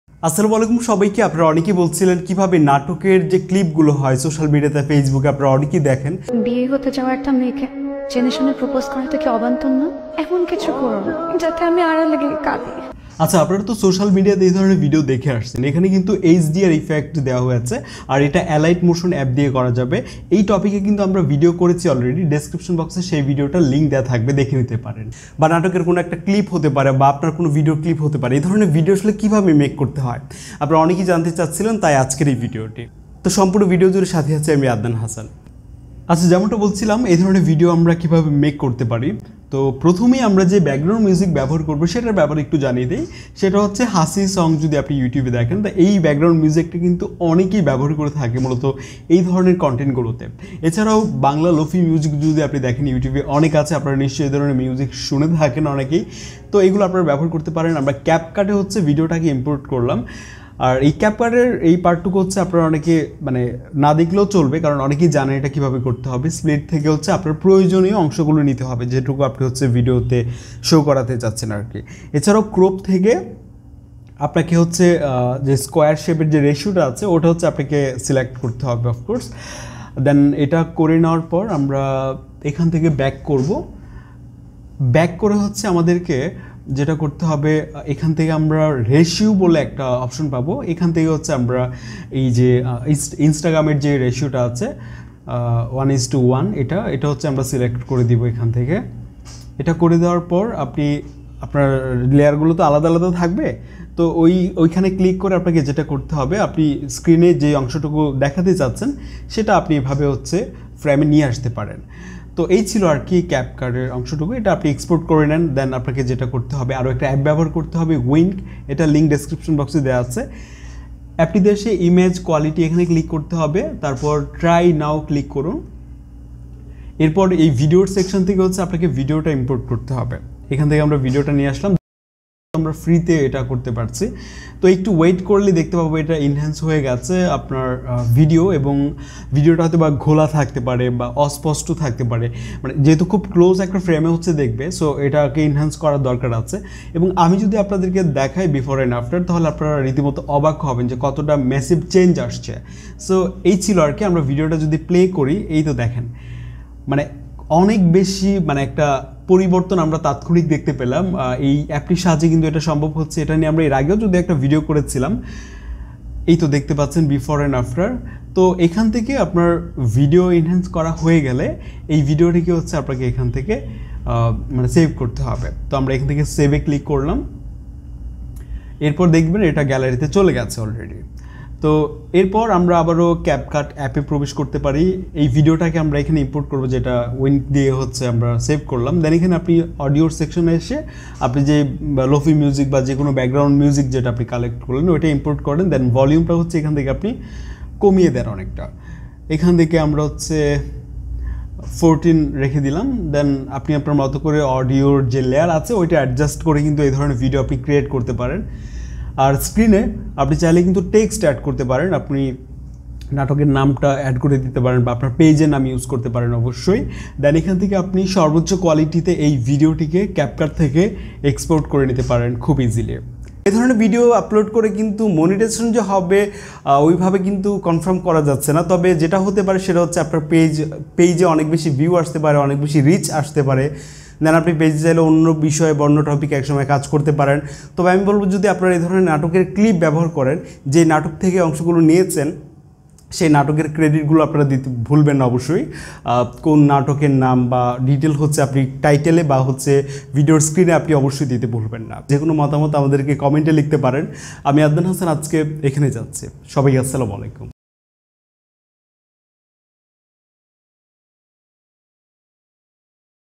ফেসবুকে এই ধরনের ভিডিও আপলোড করে ইনকাম? Edit bangla Natok status video & HDR Effect | TECH BONGO
Many of you have been asking for such a tutorial and to share some more details about this topic so hope today's video will be helpful for you.